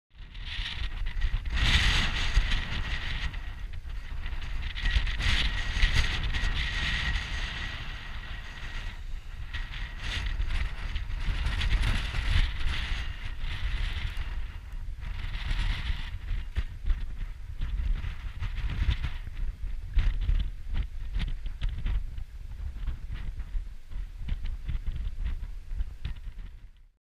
Komna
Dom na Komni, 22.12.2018

Malce vetrovno v okolici doma.